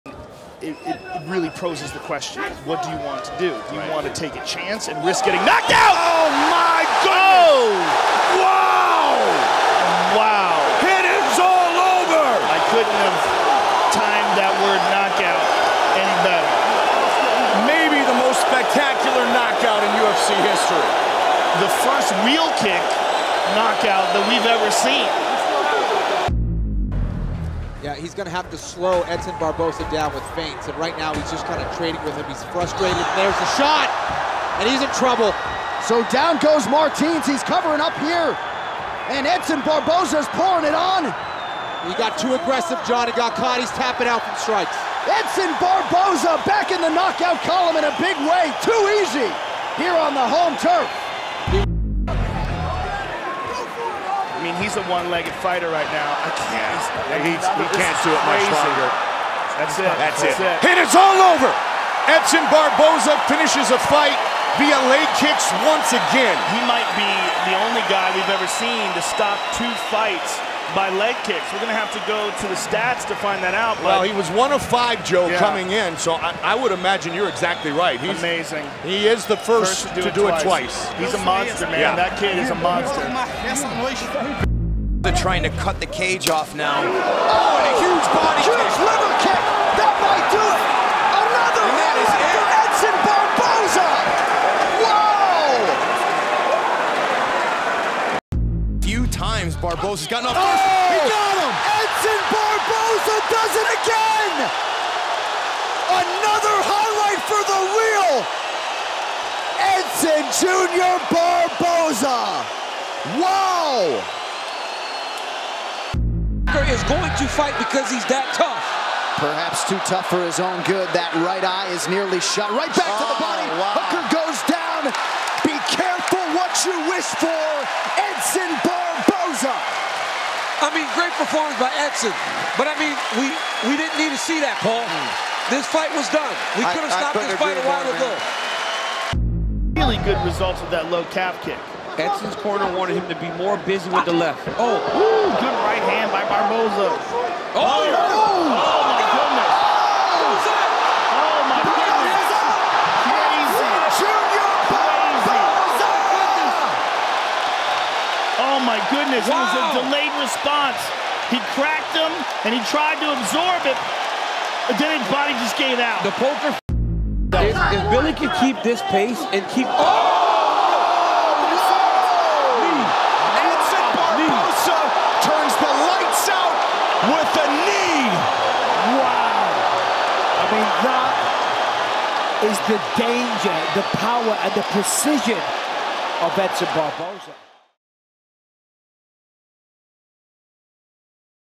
Eight Reasons Why Edson Barboza is a Certified Octagon Legend
Since his Octagon debut over a decade ago, featherweight Edson Barboza has not slowed down, continuing to challenge himself against some of the divisions best while accumulating a 17-11 record in the Octagon. Barboza is back in action on Saturday as he faces Sodiq Yusuff in the UFC Vegas 81 main event.